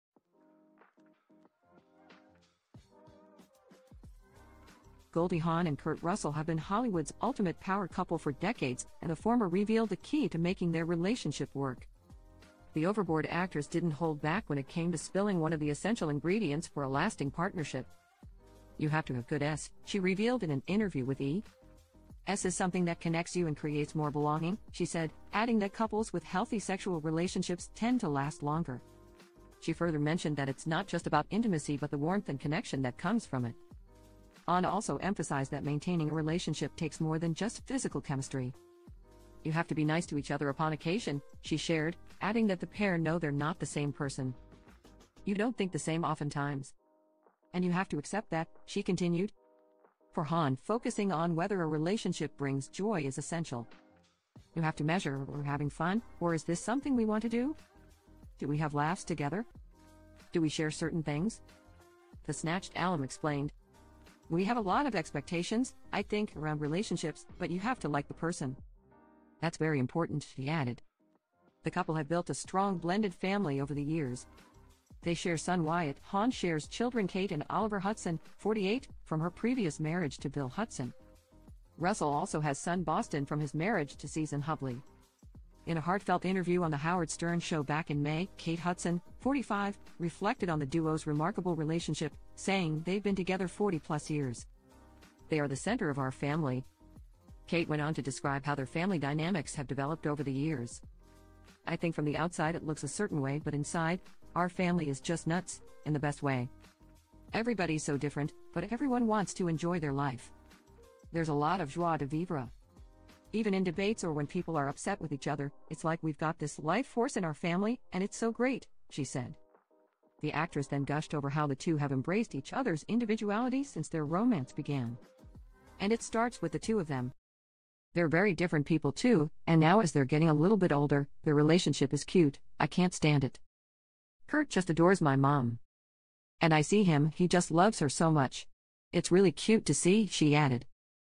Goldie Hawn Reveals the Key to Her Long-Lasting Relationship With Kurt Russell
Goldie Hawn and Kurt Russell have been Hollywood's ultimate power couple for decades, and the former revealed the key to making their relationship work. "S-- is something that connects you and creates more belonging," she said, adding that couples with healthy sexual relationships tend to last longer. In a heartfelt interview on The Howard Stern Show back in May, Kate Hudson, 45, reflected on the duo's remarkable relationship, saying, "They've been together 40 plus years. Even in debates or when people are upset with each other, it's like we've got this life force in our family and it's so great," she said. They're very different people too, and now as they're getting a little bit older, their relationship is cute, I can't stand it.